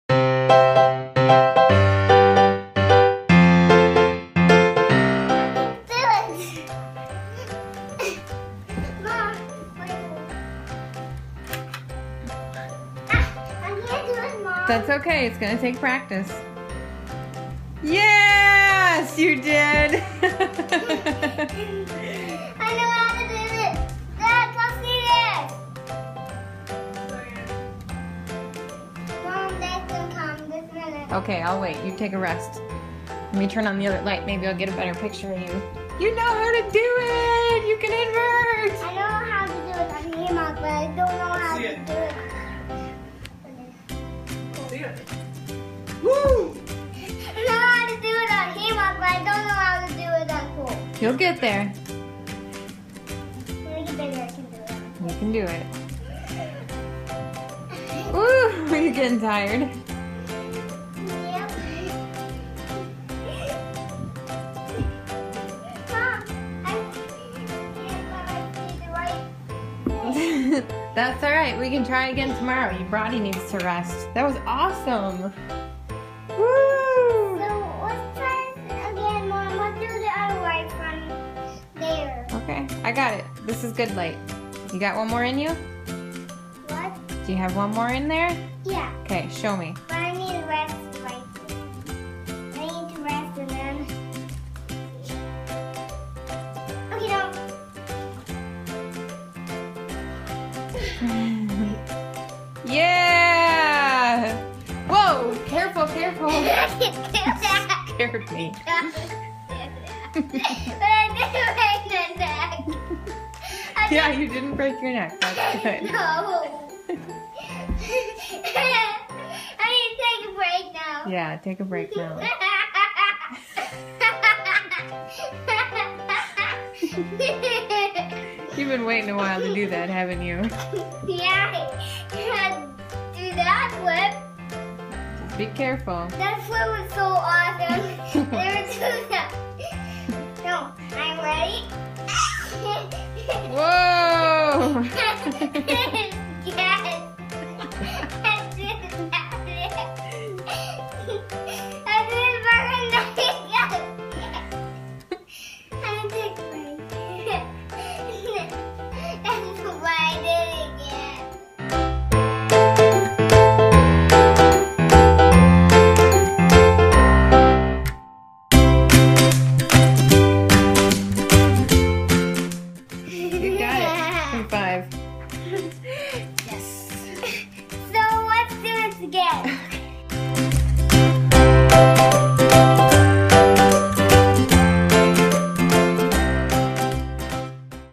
My Son's First Invert!!
This happened about three weeks ago (I am behind on my video editing) SOOOO excited for him :P